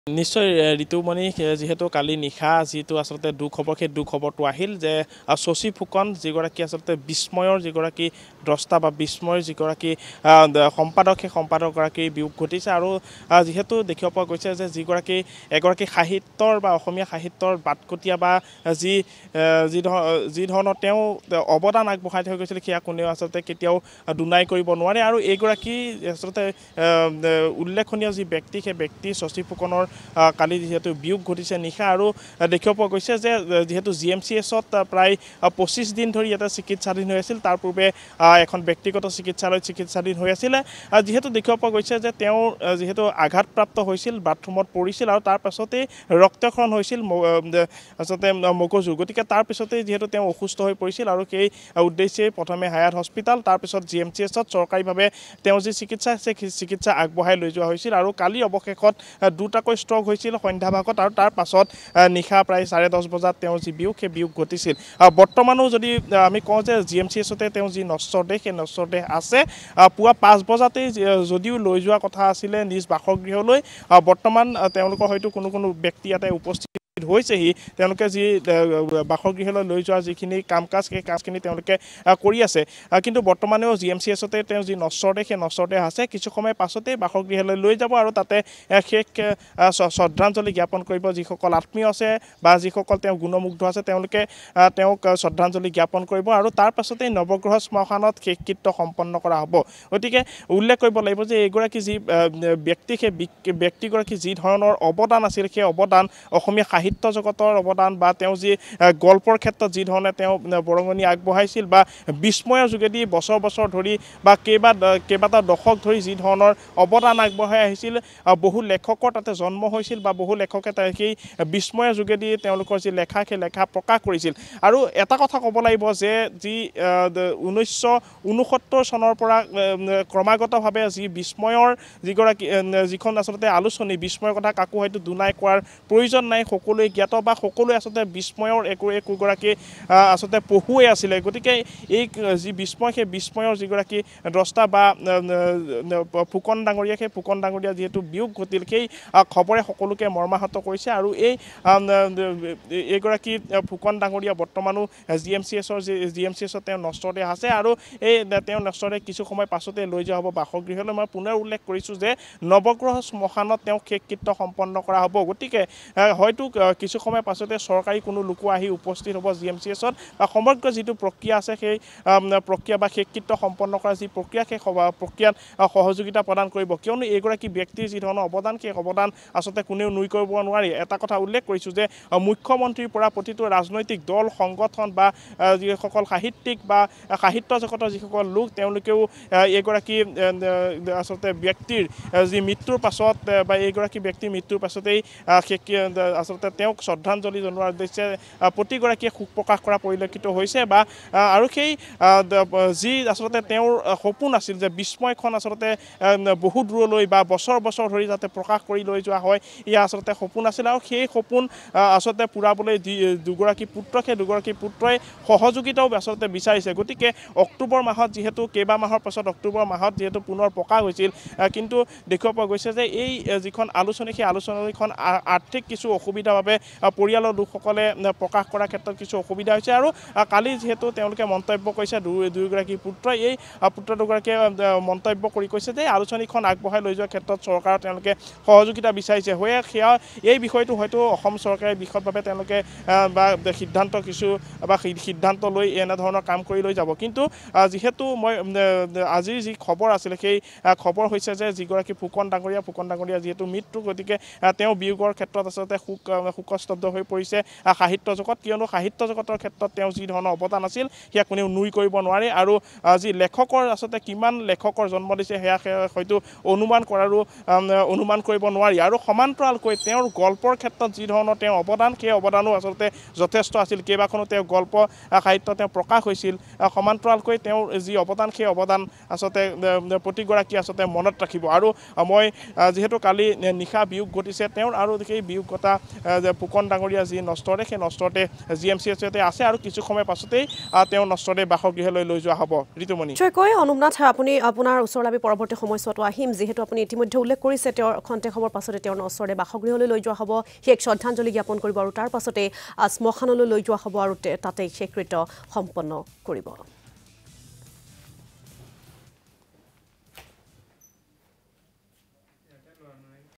‘বিস্ময়’ৰ প্রকাশক তথা প্রতিষ্ঠাপক সম্পাদক শশী ফুকনৰ মৃত্যু,নিশা GMCHত শেষ নিশ্বাস ত্যাগ ফুকনৰ ।
‘বিস্ময়’ৰ প্রকাশক তথা প্রতিষ্ঠাপক সম্পাদক শশী ফুকনৰ মৃত্যু,নিশা GMCHত শেষ নিশ্বাস ত্যাগ ফুকনৰ ।GMCH ৰ পৰা আমাৰ এক প্ৰতিবেদন